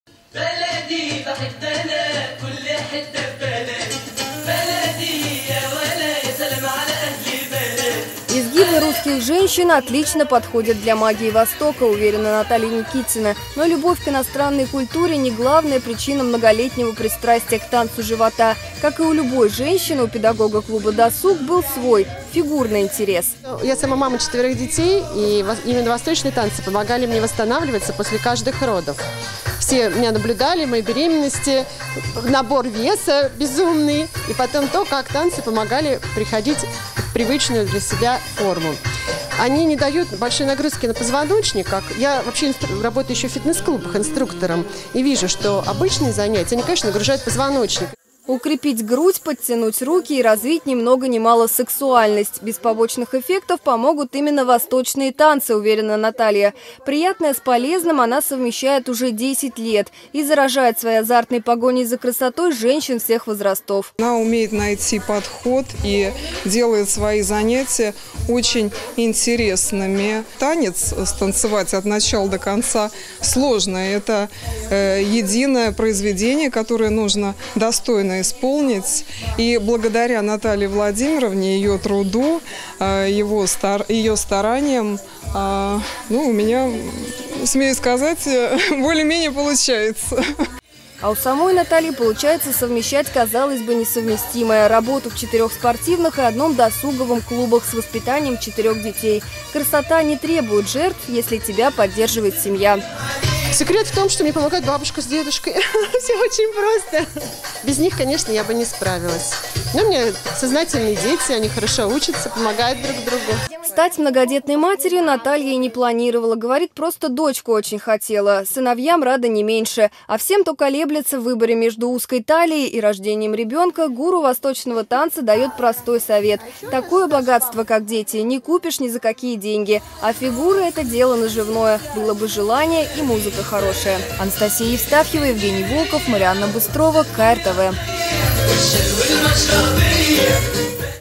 Танец живота творит чудеса
Новости. 28 ноября

В клубе Досуг танец живота преподает замечательный педагог с многолетним стажем Наталья Никитина. Глядя на ее фигуру и не скажешь, что она многодетная мать. Как удается совмещать эти две профессии, узнаем из следующего сюжета.